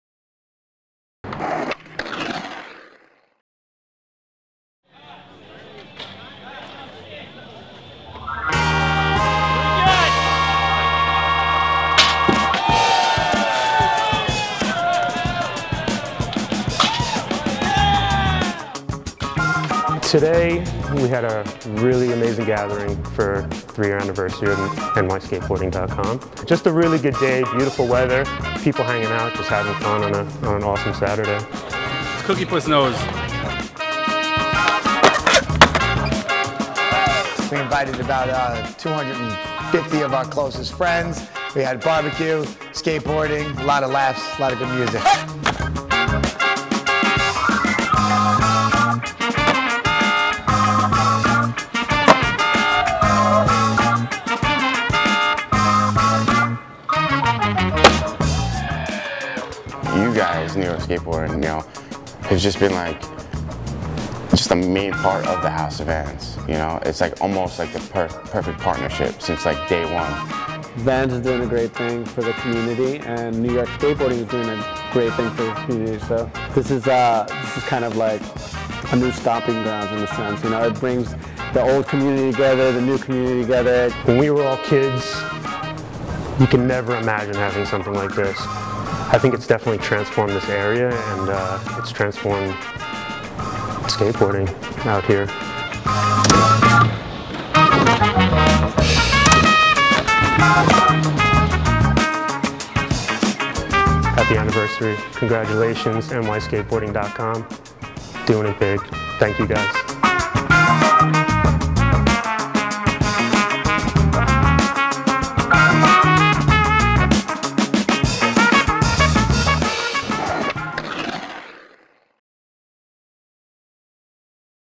NYSkateboarding's 3 Year Party (2012)
NYSkateboarding.com celebrates 3 years at the House of Vans in Brooklyn, NY.

Edited by Michael Hurley
Additional filming by Dennis Williford